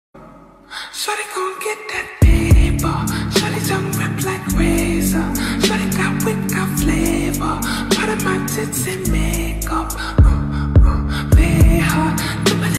Trust the process!❣️
"An abstract painting showing the contrast between the beauty of peacock and brown beautiful girl!"❣️✨ browngirl peacock clash # beauty